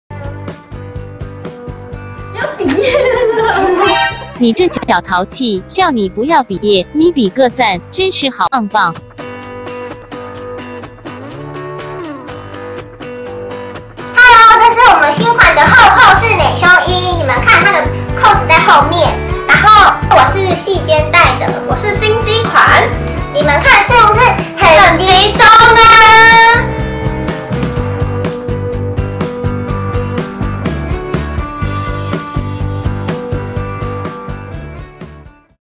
青春活力兩人組~ 迸出什麼搞笑情節呢??!
請開聲音~
淘氣奕瑜又幹了什麼好事😍
讓大家哭笑不得😀
點進來看看吧😝😝
奕瑜跟佳雯身上的是E-HEART機能型美胸衣😳
無鋼圈卻還是如此集中💪
本身布料有些緊度(不是那種鬆鬆、很有彈性的布料)👀
可以幫助胸型集中托高❤❤
不走山!!👌👌